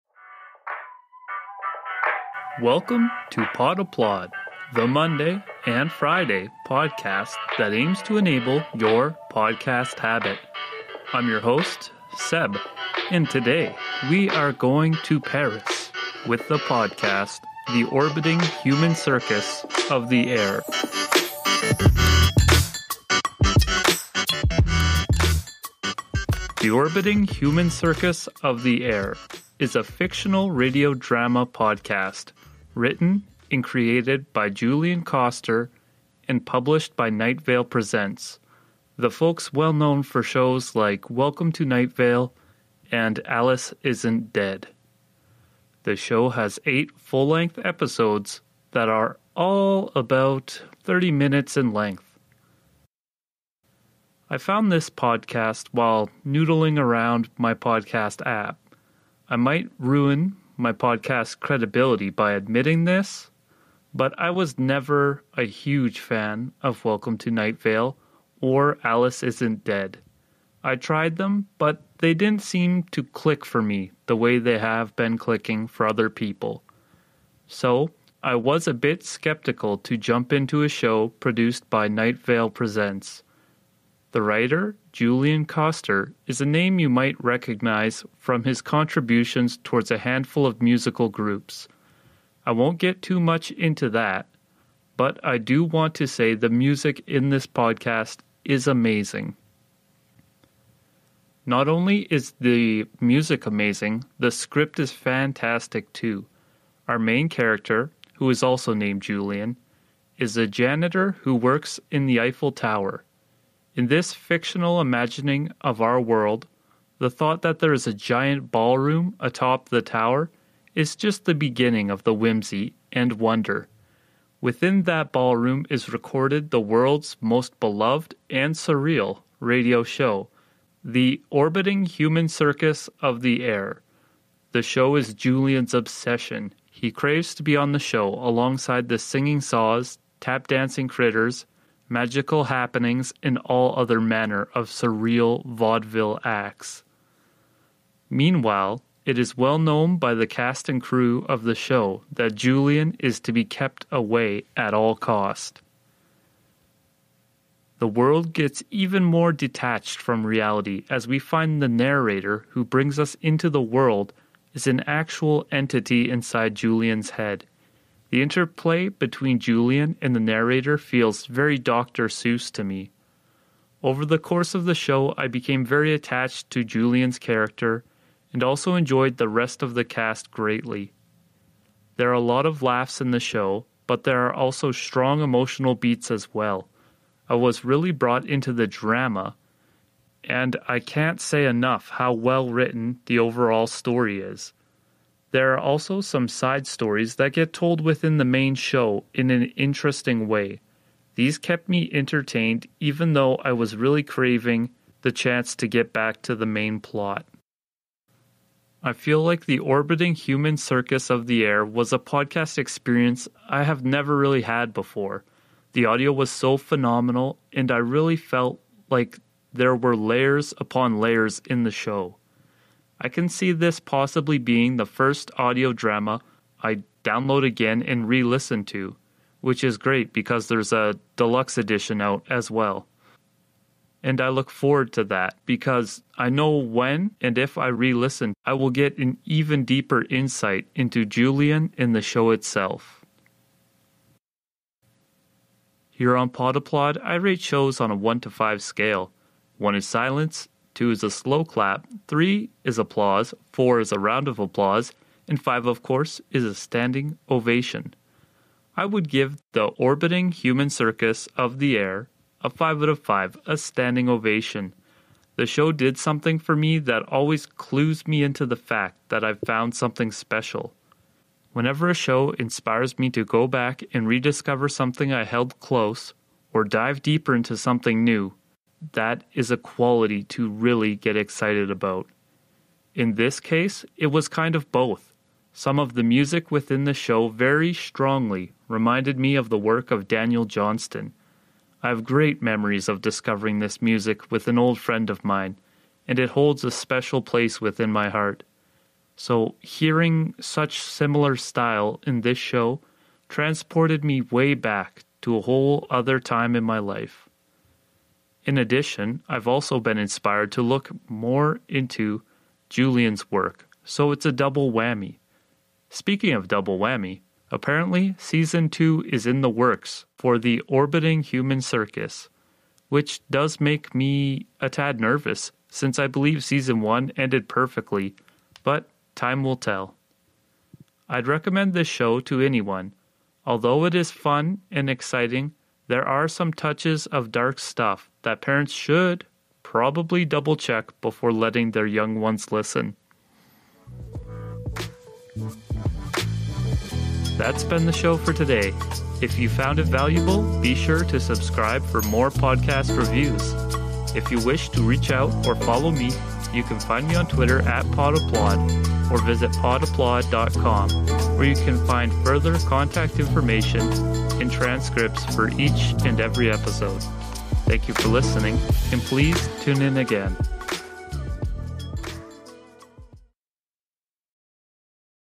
The Orbiting Human Circus - Is It For You?
Is this episode I have my head in the clouds as I listen to The Orbiting Human Circus (of the air). The show brings a lot of talent to the table as a co-presentation of WNYC Studios and Night Vale Presents.

   --- New episodes upload Mondays&amp;Fridays, tune into my short 5 minute reviews to save you time and find podcasts you'll love.